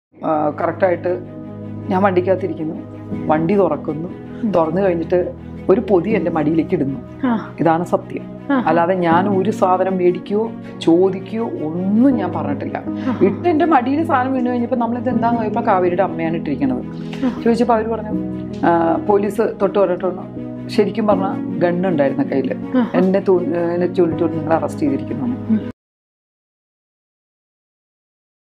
നടി കാവേരിയുമായിട്ടുള്ള യഥാർത്ഥ പ്രശ്നം എന്താണന്ന് വെളുപ്പെടുത്തി നടി പ്രിയങ്ക അനൂപ്
മുഴുവൻ വീഡിയോ കാണുന്നതിനായി ലിങ്കിൽ ക്ലിക്ക് ചെയ്യുക